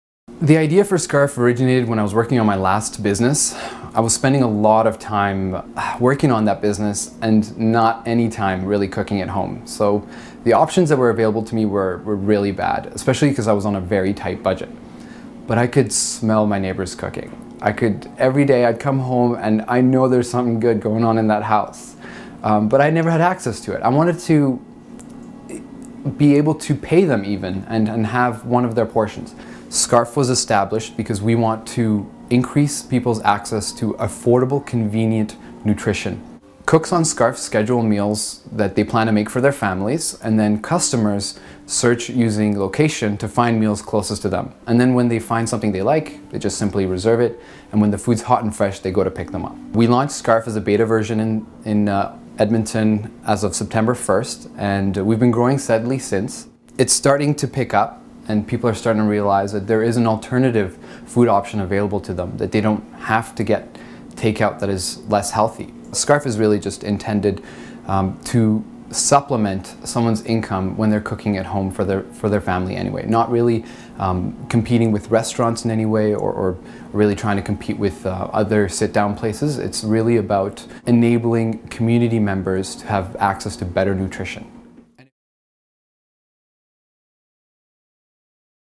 Scarf food sharing company
Scarf is an on-line business that pairs home cooks with people who want to buy home cooked meals. Video by David Bloom